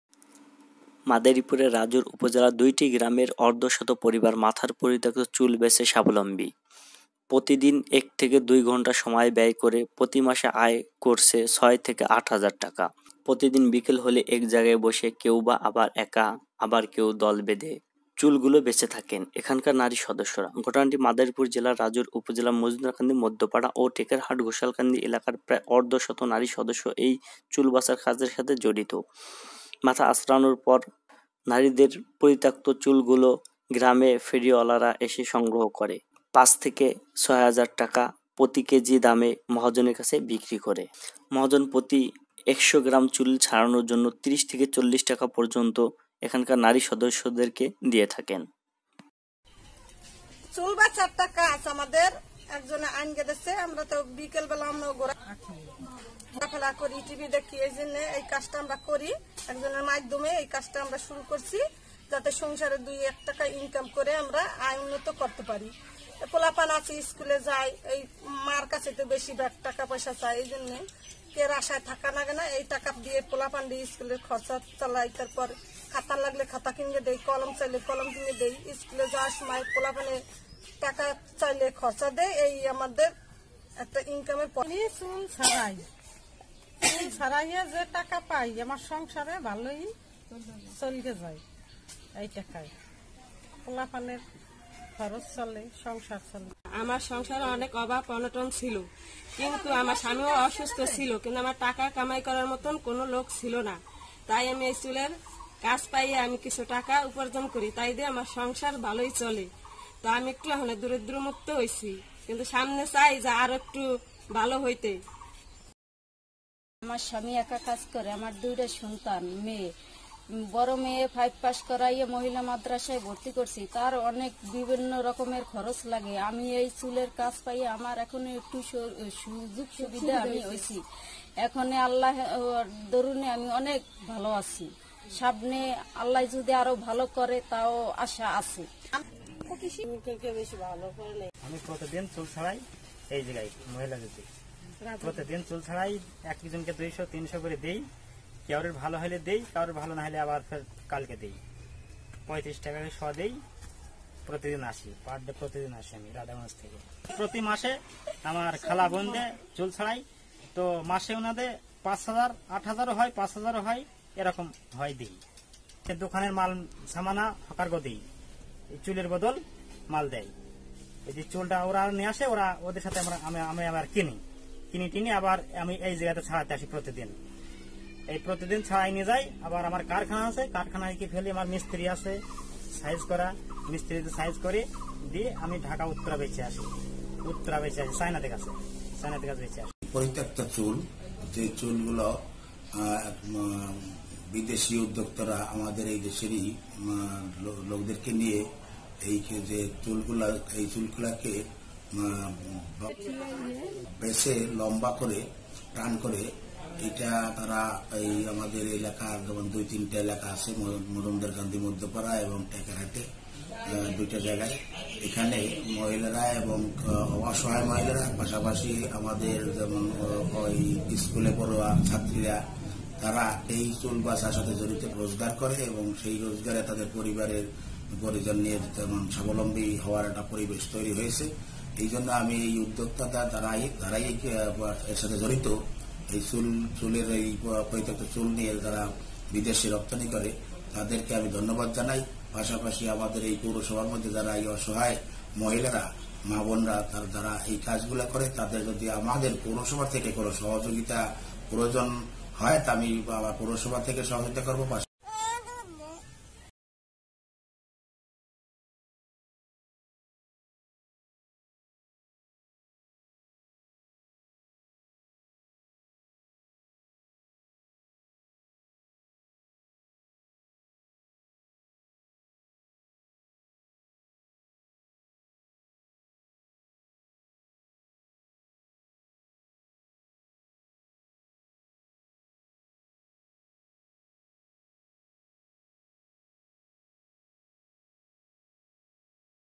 মাথার চুল দিয়ে সাবলম্বী অনেক নারী Earn money on hair base || Setu Tv
Half a hundred families choose hair চুল বেছে  স্বাবলম্বী  অর্ধশত পরিবার
Every afternoon, when sitting alone in a place, or a group of people sitting in one place and choosing the hair of the story, the women members here. . The incident is in Madaripur
About half a dozen women members of Majumderkandi Midpara and Takerhat Ghoshalkandi areas of Rajair upazila of the district are involved in this hair removal.
প্রতিদিন বিকেল হলেই এক জায়গায় বসেকেউ  একা একা আবার কেউ দল বেঁধে এক জায়গায় বসে গল্পের ছলে চুল বেছে থাকেন এখানকার নারীরা সদস্যরা। ।  ঘটনা টি মাদারীপুর
জেলার রাজৈর উপজেলার মজুমদারকান্দি মধ্যপাড়া ও টেকেরহাট ঘোষালকান্দি এলাকার প্রায় অর্ধশত নারী সদস্যরা এই চুল বাছার(ছাড়ানো) কাজের সাথে জড়িত।মাথা আঁচরানোর পর বা মহিলাদের পরিত্যক্ত চুলগুলো গ্রামে গ্রামে ফেরিওয়ালারা গিয়ে সংগ্রহ করে ৫ থেকে ৬ হাজার টাকা প্রতি কেজি দামে মহাজনের কাছে বিক্রি করে।